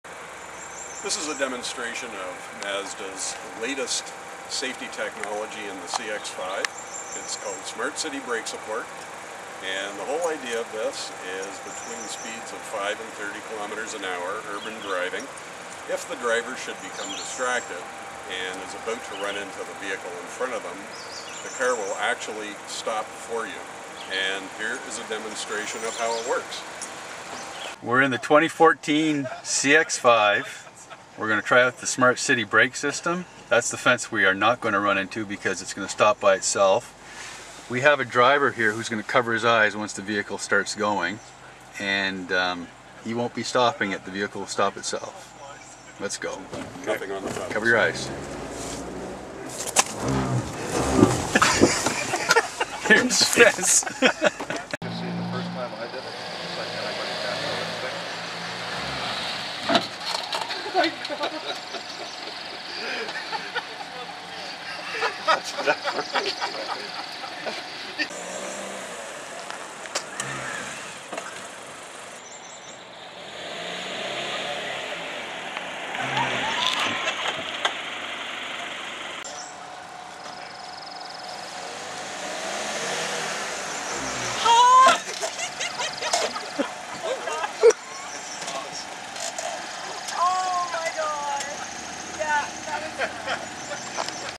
Crash Averted with Mazda Smart City Brake Support
Mazda CX5 with the smart city brake support demonstrated at Fredericton Mazda. Also available in GT Tech trim level of the Mazda 3, Mazda 6 and CX3. This systems detects objects ahead at low speeds. If no driver action is taken, the system automatically applies the brakes.